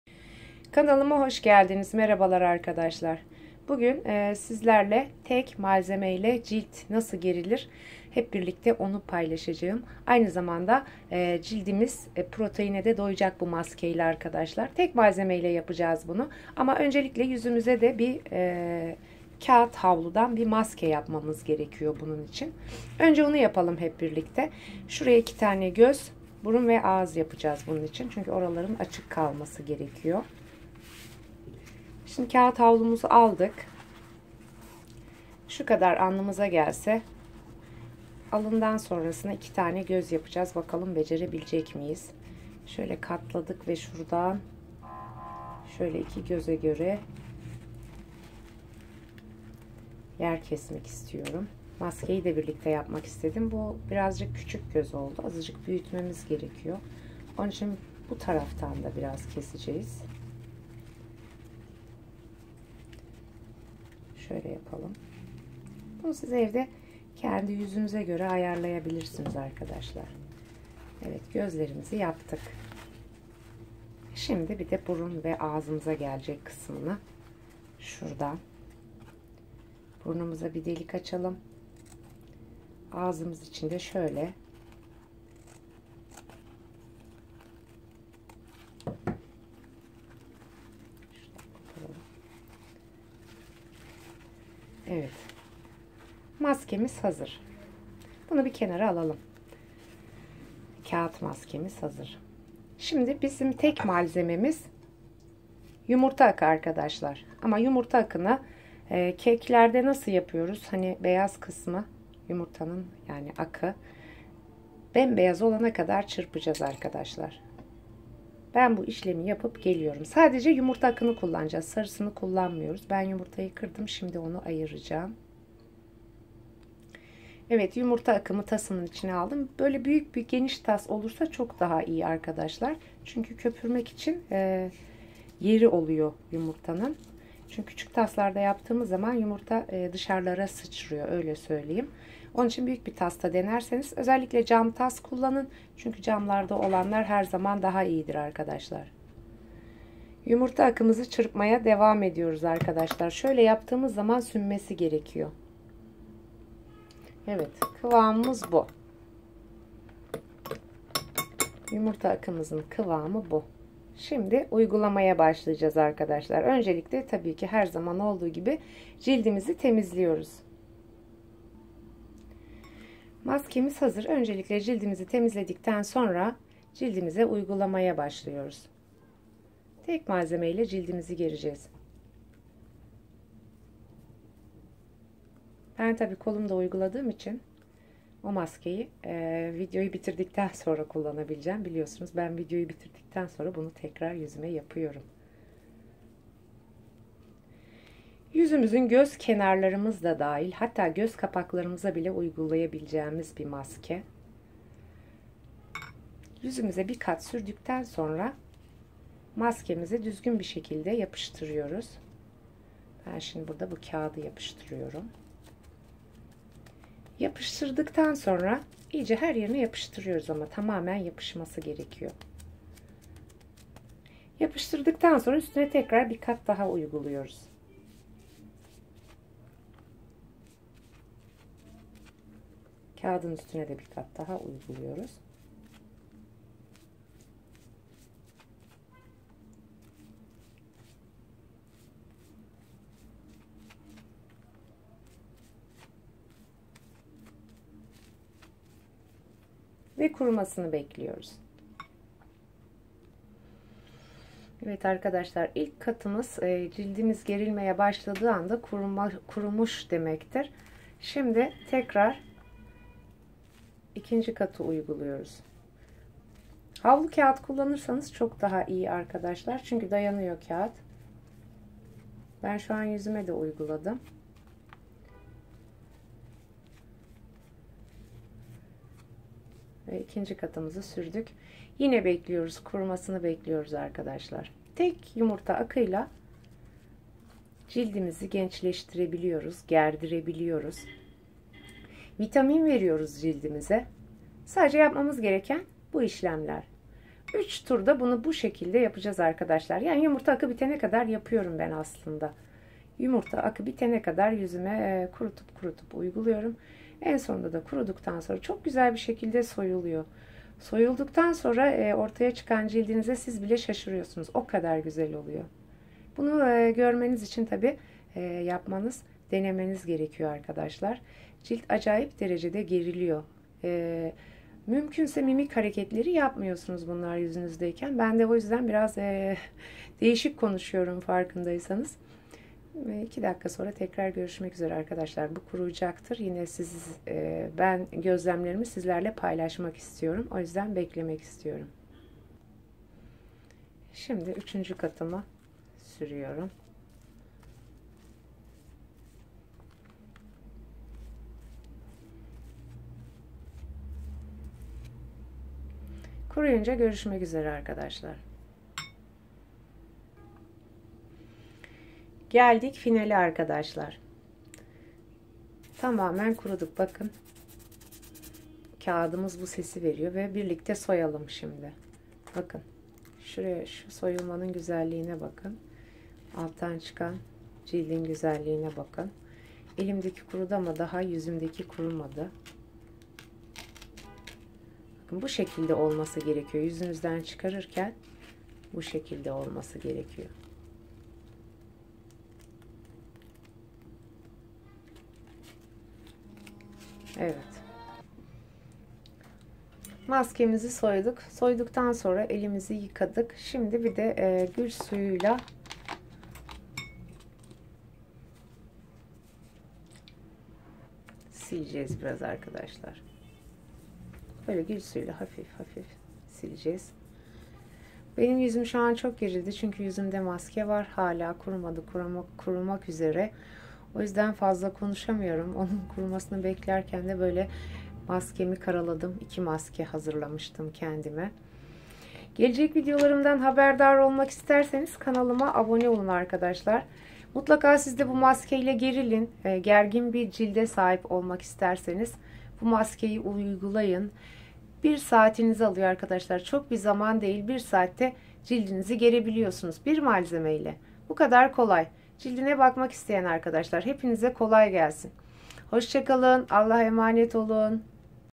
Evde Yüz Germe -TEK MALZEME - Yumurta Akı Maskesi -BOTOKS ETKİLİ- Doğal-Güzellik- Bakım
MERHABA ARKADAŞLAR
Bu video da evde yüz germe,tek malzeme,kullanarak,yumurta
 akı maskesi, ni uyguladım. Botoks etkili, maske.Güzellik, bakım için ,doğal olarak,evde yüz germe,nasıl yapılır,yöntem ini paylaştım.
iyi seyirler,

Sizde cildinizi germek isterseniz mutlaka deneyin.Asla pişman olmucaksınız.Deneyen arkadaşlar mutlaka yorumlarınızı benimle paylaşırmısınız.Bu gerilme hissi insana genç hissettiriyo.Genç hissetmek genç görünmekten daha güzel gibi sanki🤗😊😊🌷🌷💖💖